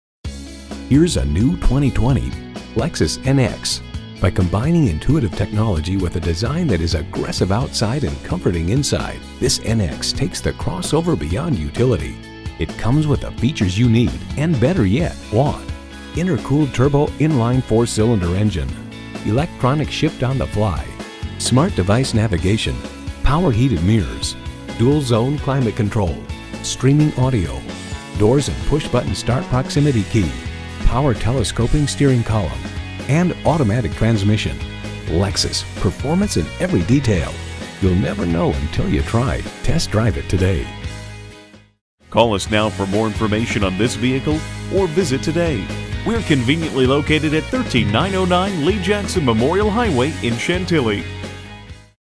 New 2020 Lexus NX Chantilly Dale City VA DC, MD NXL2218131 - SOLD
Year: 2020
Make: Lexus
Model: NX
Trim: NX 300 AWD
Engine: 122 L 4 Cyl. Cyl.
Color: Cadmium Orange
Interior: Black
Stock #: NXL2218131
VIN: JTJAARDZ1L2218131

Address:
13909 Lee Jackson Memorial Hwy